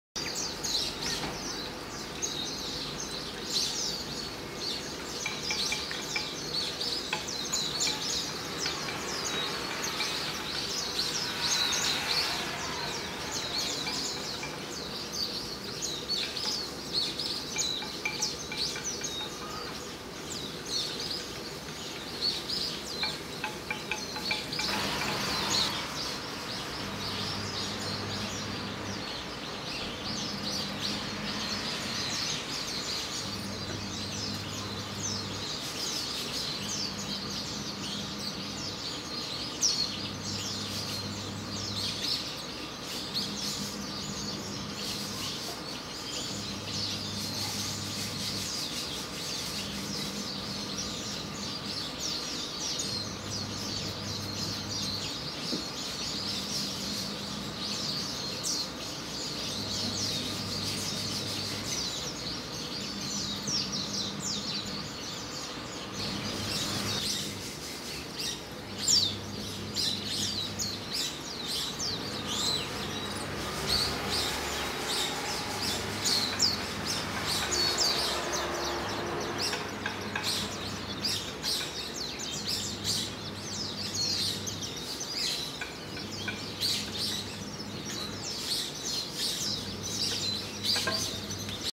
20110326 メジロ桑の実をつつく
小笠原父島のメジロです。